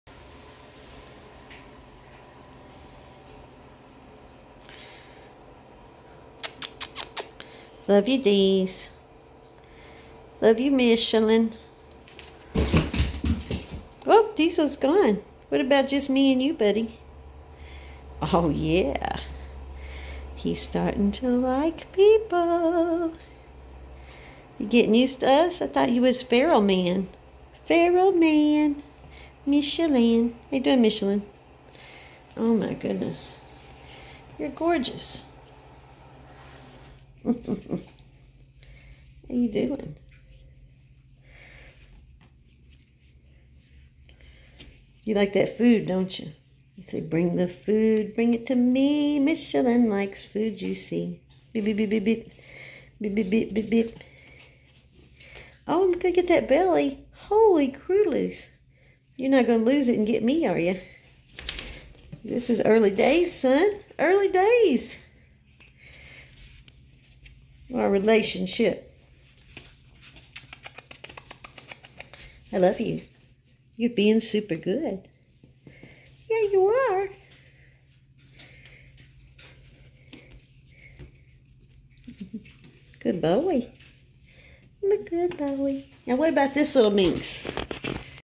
Michelin was trapped at a feral colony only a few weeks ago. Friendly now!
Clarkson, KY
Michelin was trapped a few weeks ago and was hissy and scared for a few days
...but this is a friendly cat and he needs a safe indoor home.